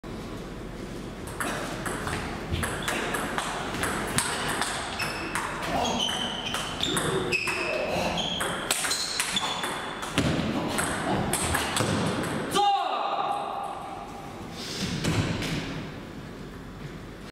Копия видео "21 августа 2018. Красный зал. TT Cup"
Лисечко А. - Шепель В.
Рева А. - Степанов Д.
Кузнецов И. - Ильющенко С.
Online table tennis series TT Cup
Watch all matches online
Your table tennis TT Cup professional series